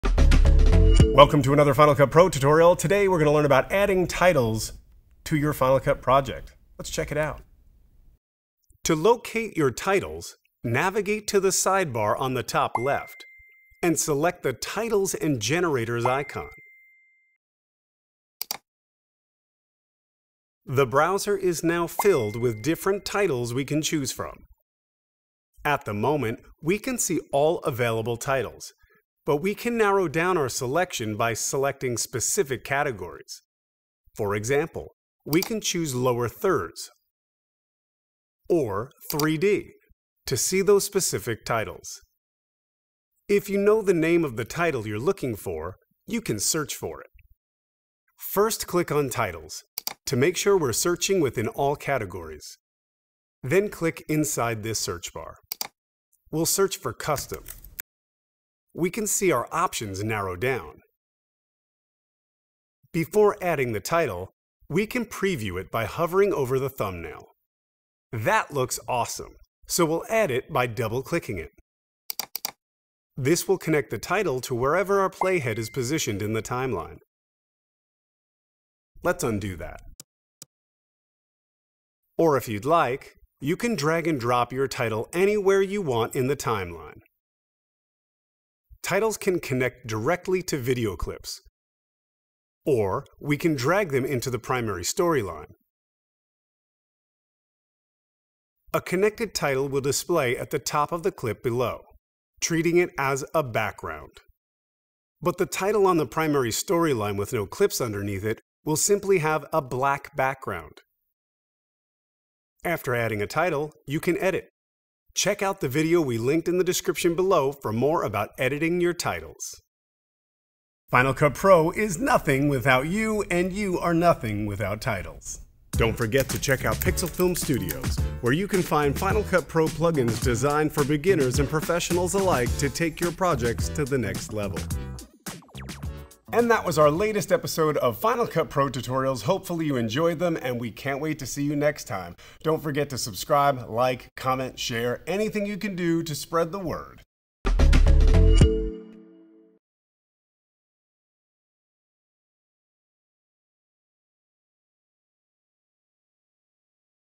How to Add Titles to Your Final Cut Pro X Projects
Titles are an essential part of any Final Cut Pro X project, delivering information on screen while your footage plays. In this video tutorial, we'll show you the basics of adding a title, such as how to navigate the titles browser, and different ways of adding a title to your timeline.

Chapters:
00:00 - Intro
00:08 - Locate Titles Menu
00:42 - Search for a Title
01:07 - Add a Title to the Timeline
02:00 - Outro